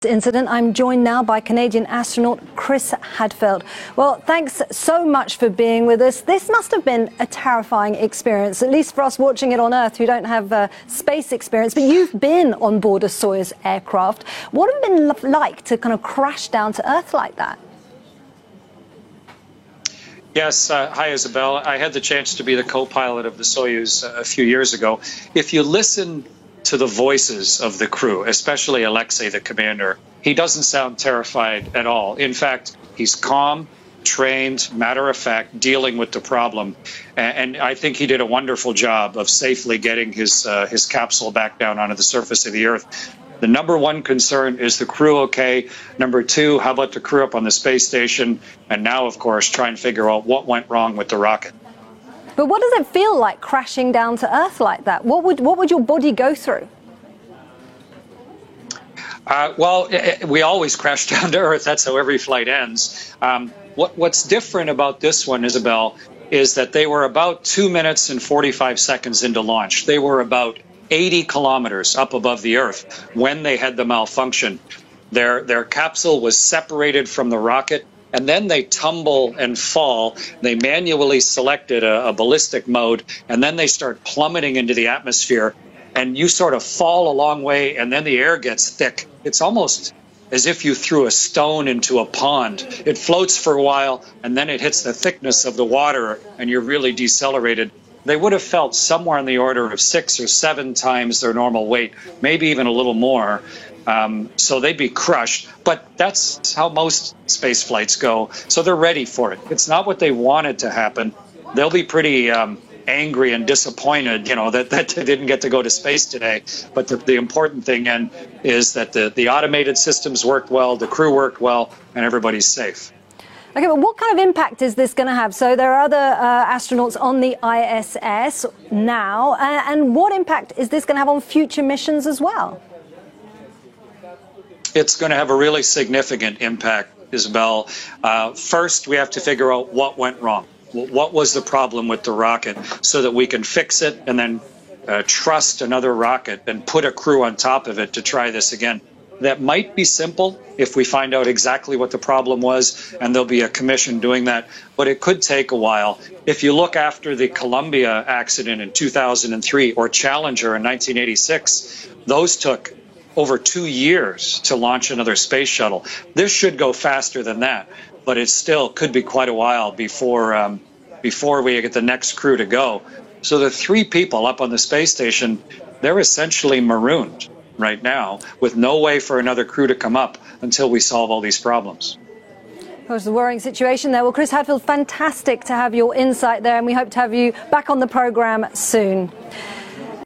Crew stuck on International Space Station 'marooned', says Chris Hadfield
Renowned Canadian astronaut Chris Hadfield says the three people stuck on the International Space Station are essentially 'marooned' there for a while. …